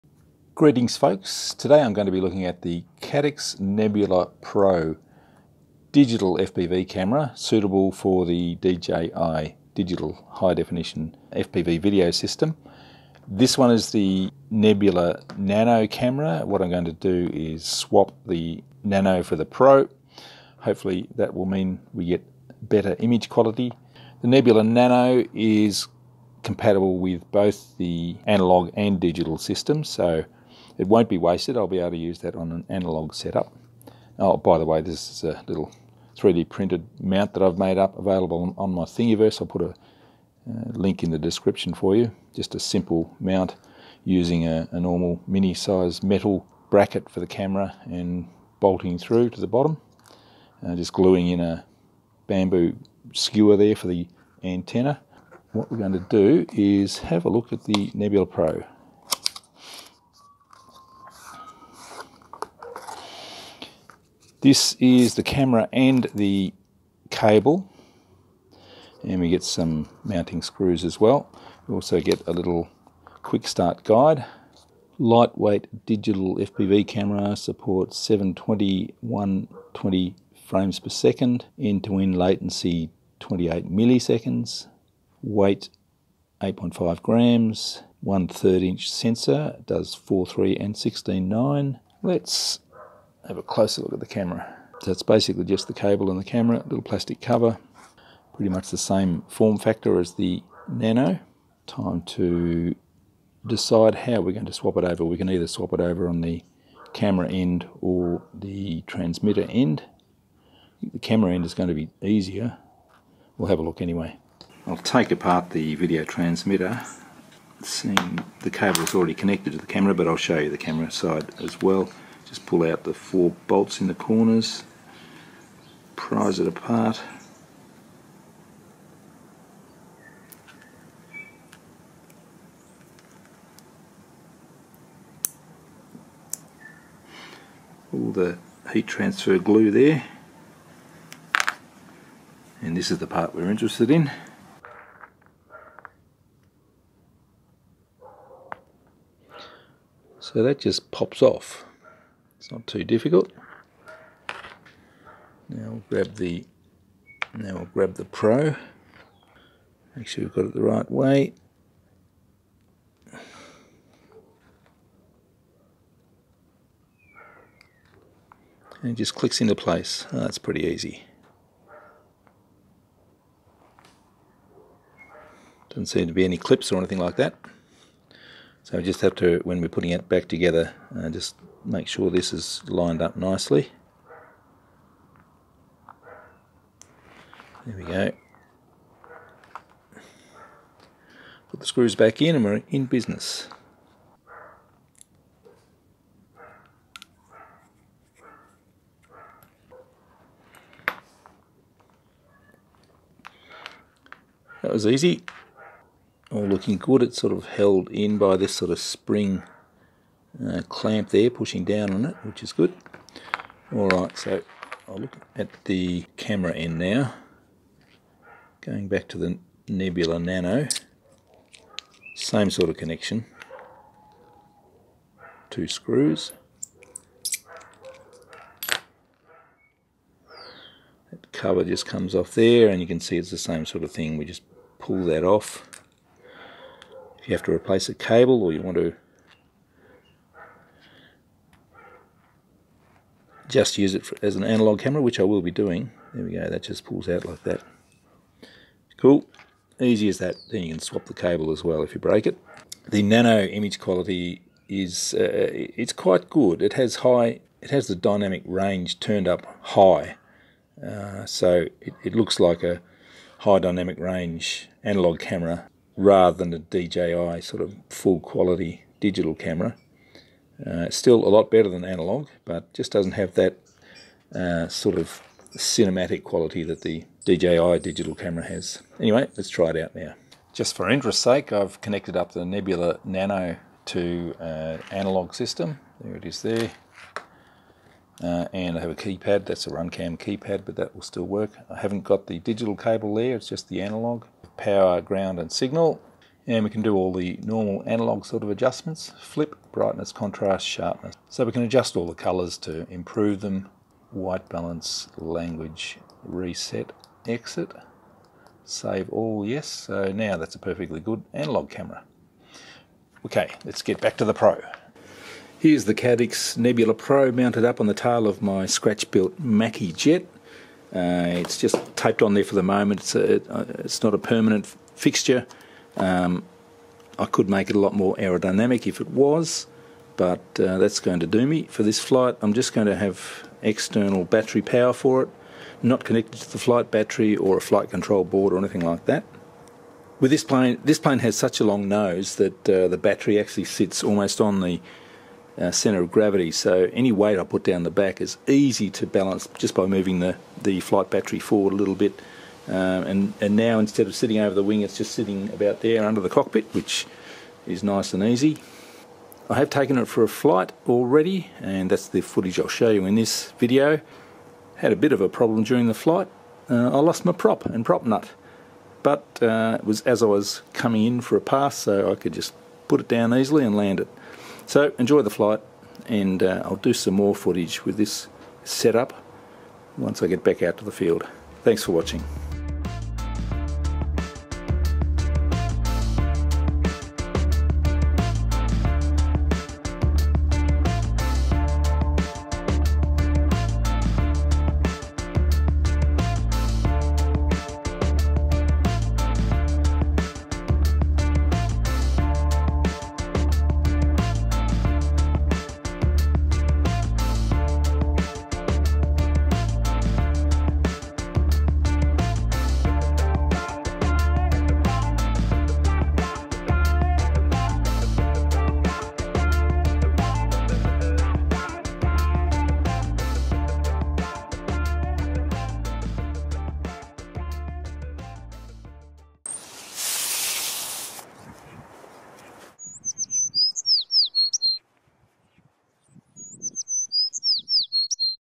Caddx Nebula Pro - Camera swap
Caddx Nebula Pro - How to swap cameras, and a brief flight demo on my scratch built Macchi Jet (not jet). Digital HD FPV comparable with DJI Digital FPV system.

► Close up workbench cam - Panasonic Lumix LX10

This is a fun hobby for me, not a money making venture.